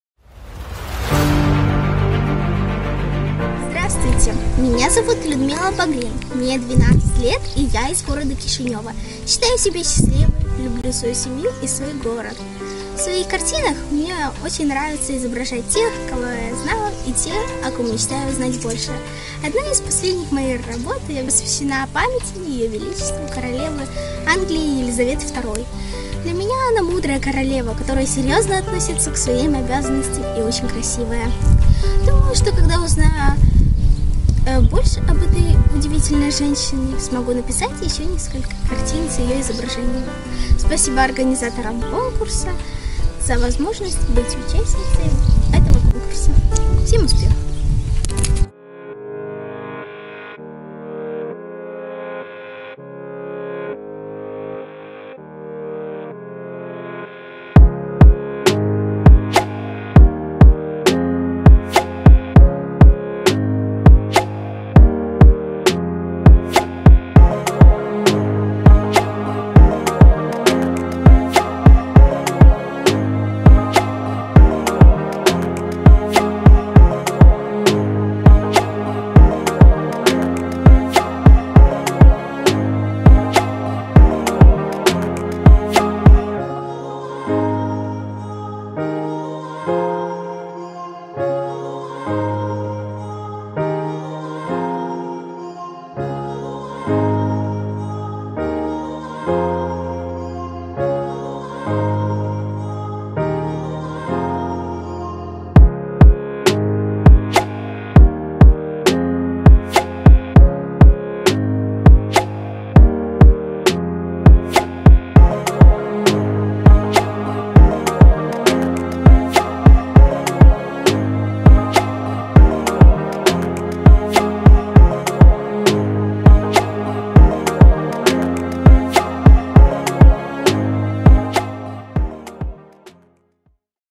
GOLDEN TIME TALENT | GRAND FINAL ART3 | Liudmila Bagrin | Graphic arts
Member/team name: Liudmila Bagrin

The country: Moldova | City: Chișinău

Nomination: Graphic arts

Name of competitive work: Королева Англии Елизавета ll

Work size: A3

Execution material: Альбом

Manufacturing technology: Карандаш

Participation in other competitions and festivals / creative achievements: Участвовала в международном конкурсе Golden Time Talent в 44 сезоне. Получила 2 первых места в номинации графический рисунок.